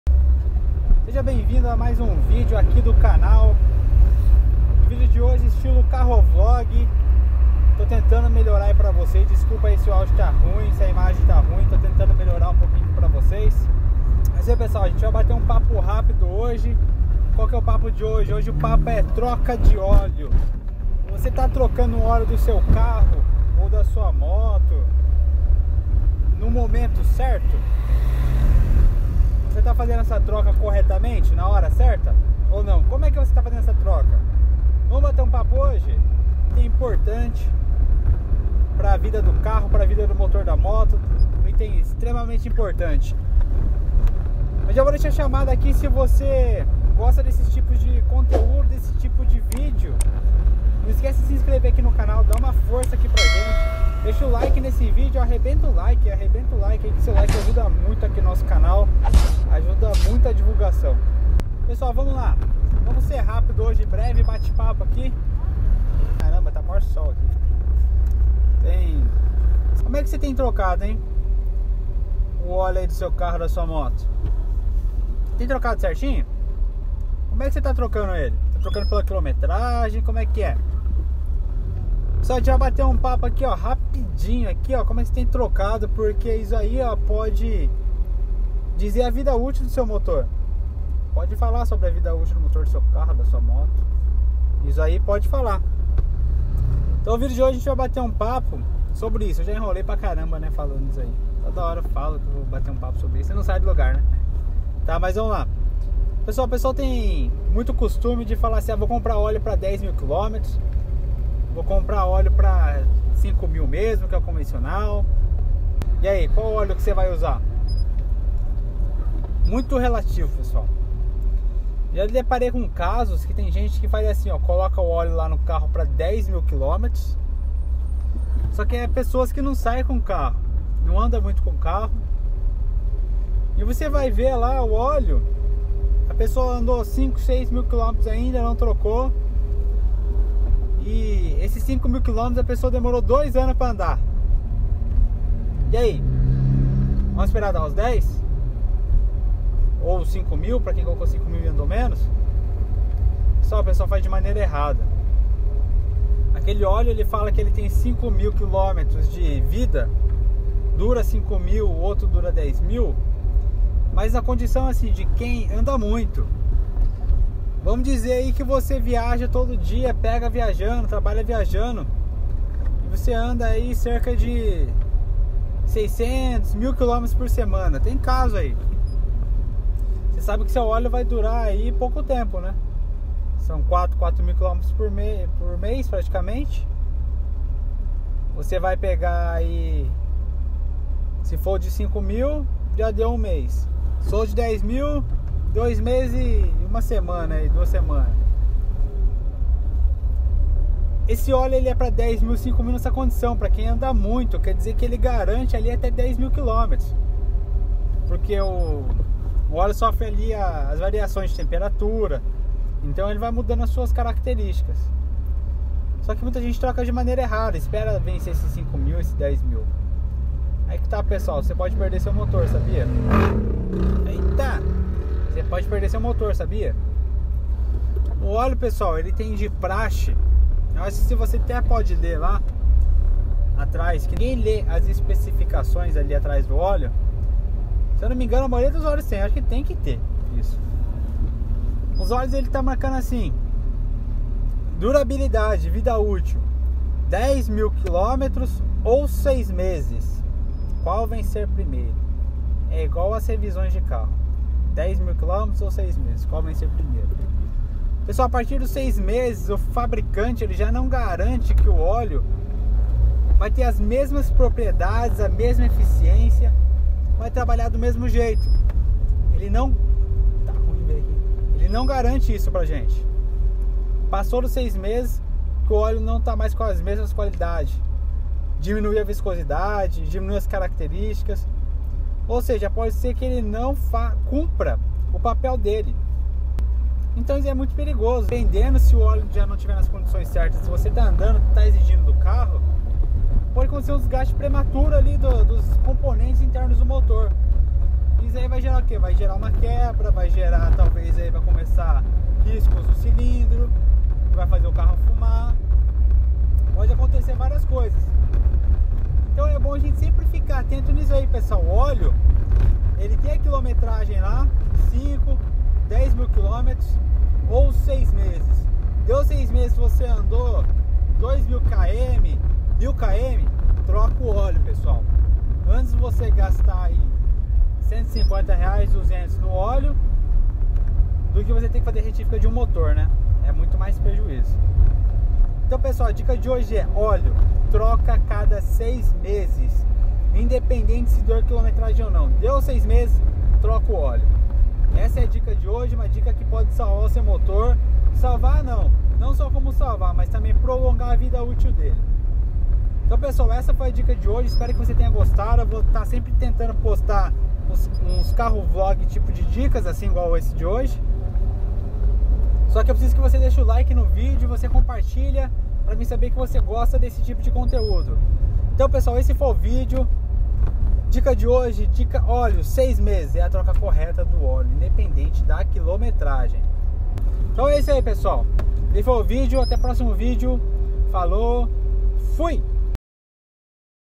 TROCAR O OLEO CORRETAMENTE - CARROVLOG
FALA RAPAZIADA!

SEJA BEM VINDO A MAIS UM VÍDEO DO CANAL!

NO VÍDEO DE HOJE, VOU TRAZER UM BATE PAPO PRA VOCÊS NO ESTILO CARRO VLOG, DANDO UMA DICA DE COMO FAZER A TROCA DO ÓLEO DO SEU CARRO CORRETAMENTE, O TEMPO CORRETO PARA SE FAZER A TROCA DO ÓLEO.

ME SIGA NO INSTAGRAM: @rafael_ag_yt